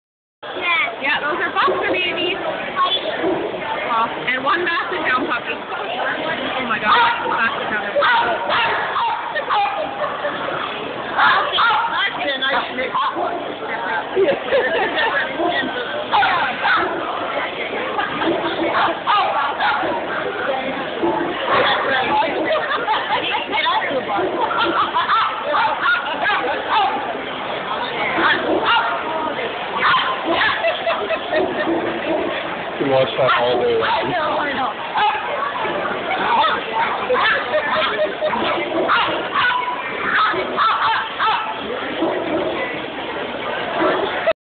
Boxer puppy and basset hound puppy
all about pets show 2010 in Toronto Ontario, Canada